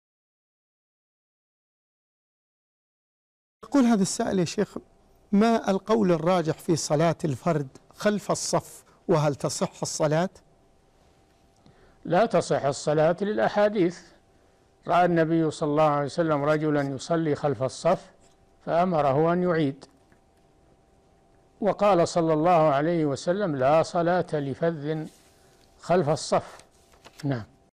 حكم الصلاة خلف الصف منفردا – الشيخ صالح الفوزان
نص السؤال: ما القول الراجح في صلاة الفرد خلف الصف وهل تصح الصلاة؟ رابط الحلقة كاملة
السيرة الذاتية :
للاشتراك في قناة الشيخ صالح الفوزان
لمشاهدة حلقات فتاوى على الهواء كاملة

رئيس الفريق
لمشروع كبار العلماء